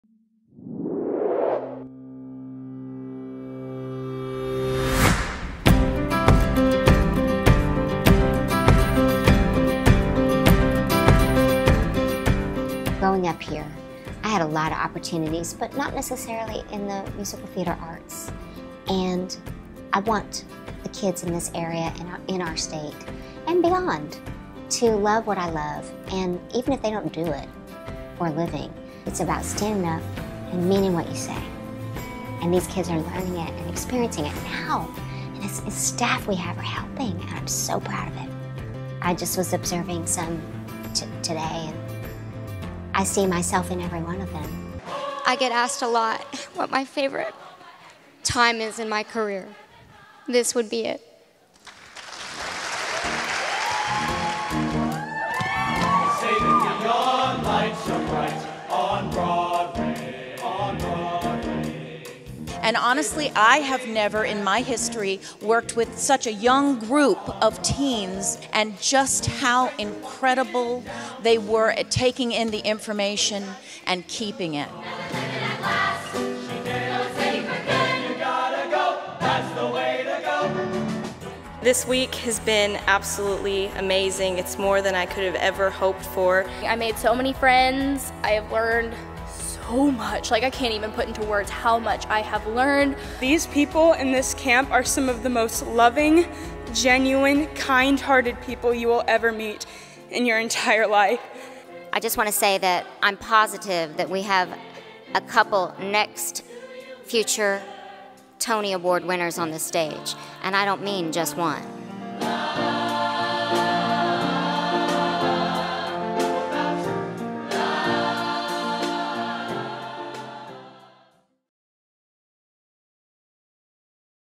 KCBBC 2021 Trailer
Kristin Chenoweth's Broadway Bootcamp is going virtual in 2021 -- featuring a robust virtual edition of the popular summer program with a stellar faculty of professionals.